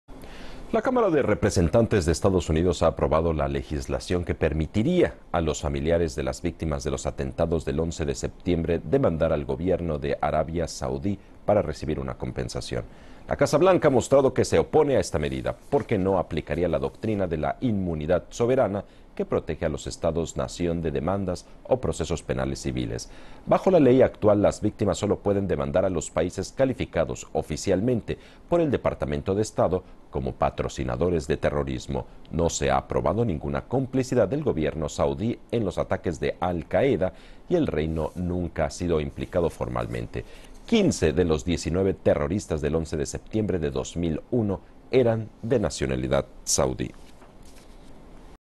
Se aprueba la ley que permitiría demandar al gobierno saudí
La Cámara de Representantes de Estados Unidos ha aprobado la legislación que permitiría a los familiaress de las víctimas de los atentados del 11 de septiembre demandar al gobierno de Arabia Saudí para recibir una compensación.  La Casa Blanca ha mostrado que se opone a esta medida, porque no aplicaría la doctrina de la inmunidad soberana que protege a los estados nación de demandas o procesos penales civiles. Bajo la ley actual, las víctimas solo pueden demandar a los países calificados oficialmente por el Departamento de Estado como patrocinadores de terrorismo. No se ha probado ninguna complicidad del gobierno saudí en los ataques de Al-Qaeda, y el reino nunca ha sido implicado formalmente. 15 de los 19 terroristas del 11 de septiembre de 2001 eran de nacionalidad saudí.